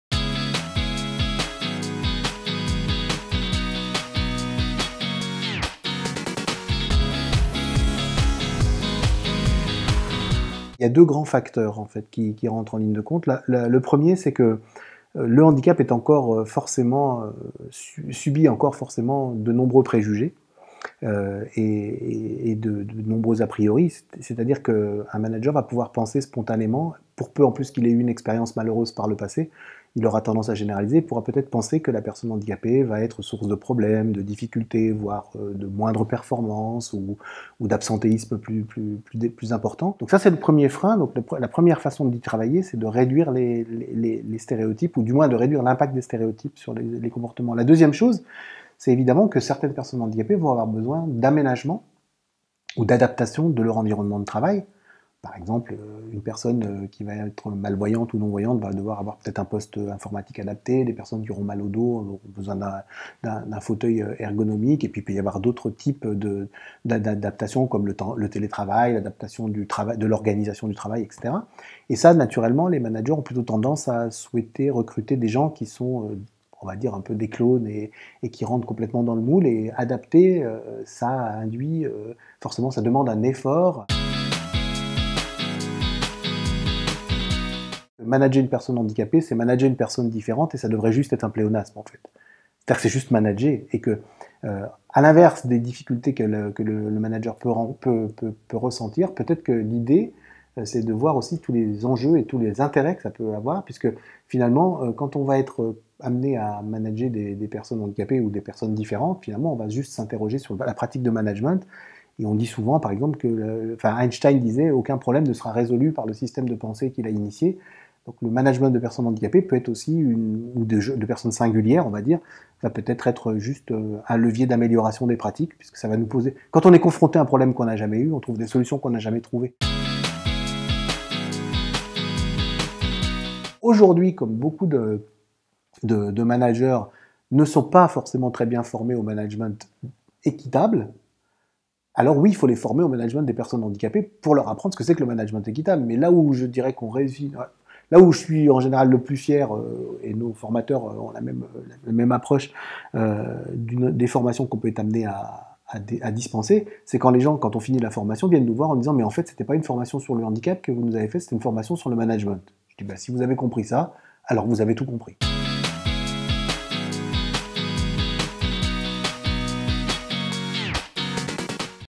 Témoignage de Guy Tisserant, président du cabinet TH conseil
Guy Tisserant forme les managers à l'intégration des personnes en situation de handicap.